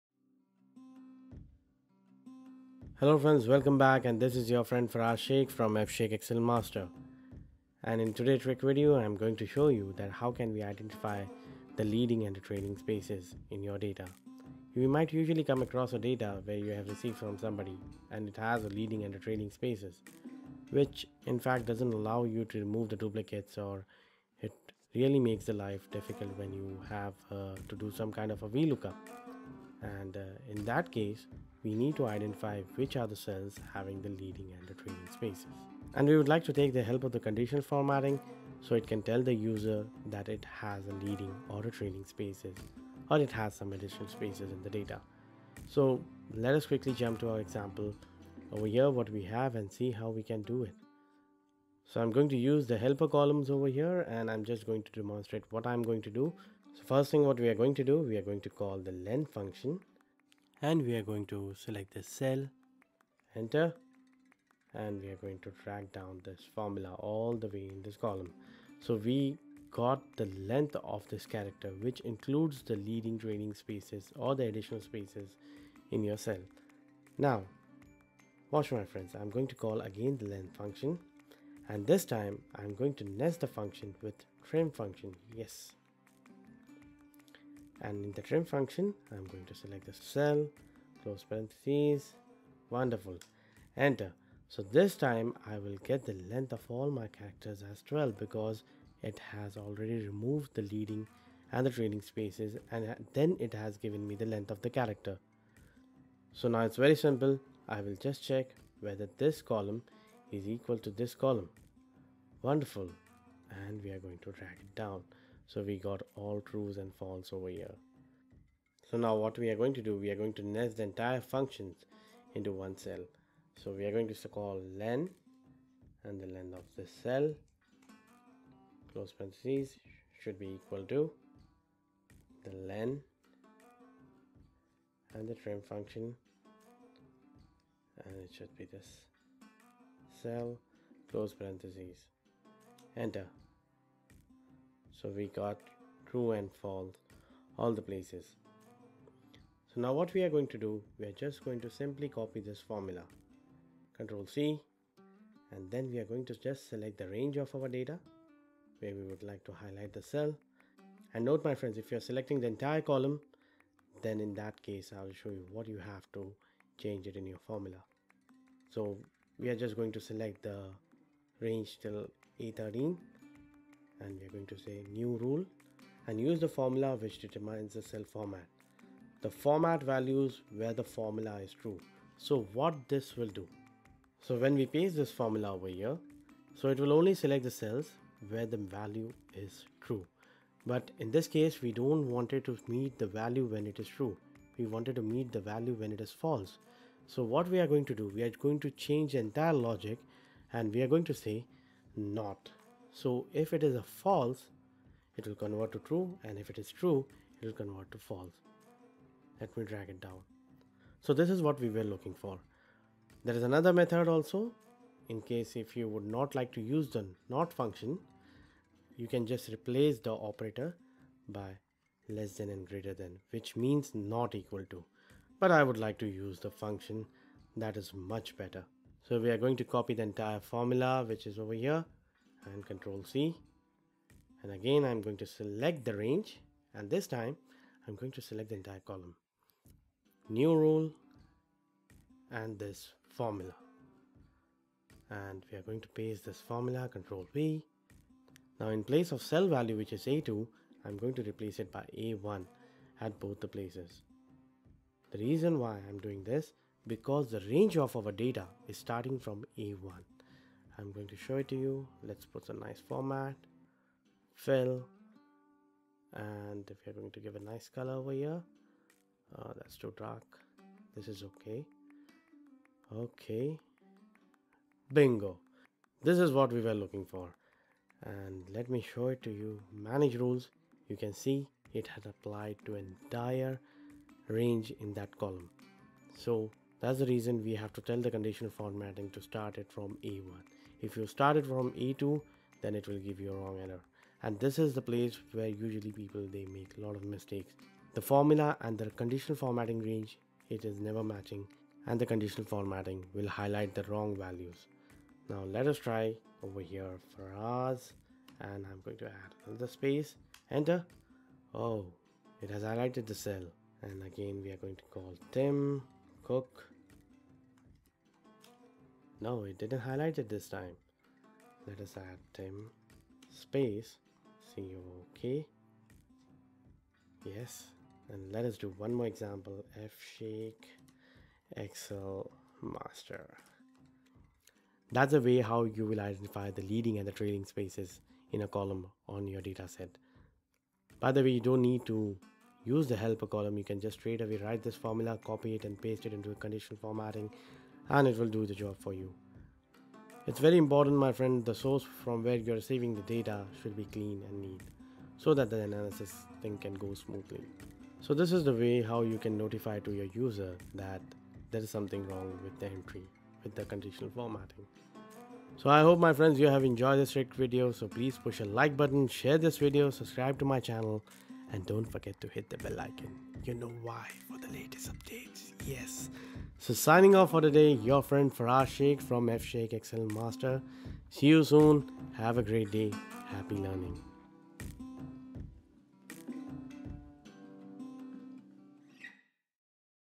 Find Leading and Trailing Spaces in Column
In this video we will learn how to find leading and trailing spaces in Column. This is very super easy to find the spaces in your data set a column and you can use this trick is for Office macOS & windows | Office365, Excel2019,2016,2013,2010

00:00 Introduction
00:14 Understanding the Scenario
00:53 Find the cell with leading and trailing spaces
05:36 End Screen